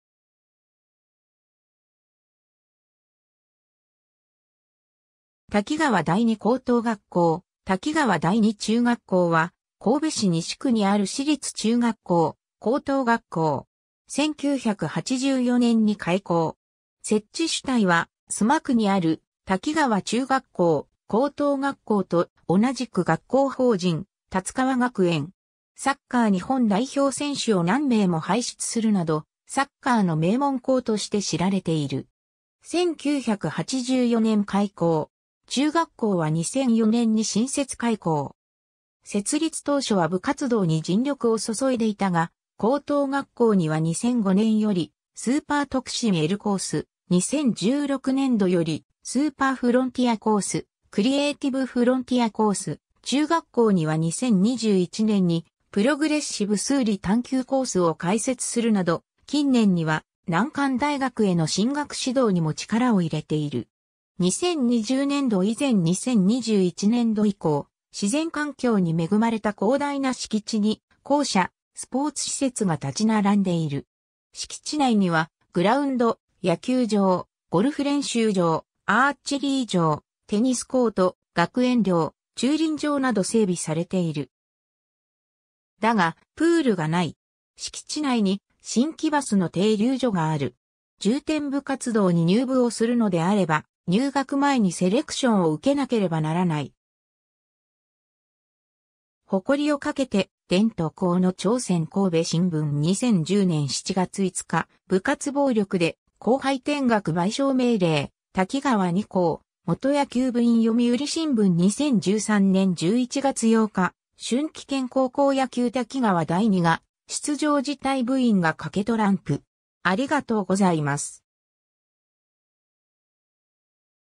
滝川第二中学校・高等学校
滝川第二高等学校・滝川第二中学校（たきがわだいにこうとうがっこう・たきがわだいにちゅうがっこう）は、神戸市西区にある私立中学校・高等学校。
 1984年に開校。
設置主体は須磨区にある滝川中学校・高等学校と同じく学校法人瀧川学園。
サッカー日本代表選手を何名も輩出するなど、サッカーの名門校として知られている。
 1984年（昭和59年）開校。
中学校は2004年に新設開校。
設立当初は部活動に尽力を注いでいたが、高等学校には2005年よりスーパー特進Lコース、2016年度よりスーパーフロンティアコース（SF）・クリエイティブフロンティアコース（CF）、中学校には2021年にプログレッシブ数理探究コースを開設するなど、近年には難関大学への進学指導にも力を入れている。
 2020年度以前 2021年度以降 自然環境に恵まれた広大な敷地に校舎、スポーツ施設が立ち並んでいる。
敷地内にはグラウンド(大・小の二つがある)、野球場、ゴルフ練習場、アーチェリー場、テニスコート、学園寮、駐輪場など整備されている。
だが、プールがない。
敷地内に神姫バスの停留所がある。
 重点部活動に入部をするのであれば入学前にセレクション(入部試験)をうけなければならない。
 ^ 誇りを懸けて 伝統校の挑戦 - 神戸新聞 2010年7月5日 ^ 「部活暴力で後輩転学」賠償命令…滝川二高・元野球部員 読売新聞 2013年11月8日 ^ 春季県高校野球　滝川第二が出場辞退　部員が賭けトランプ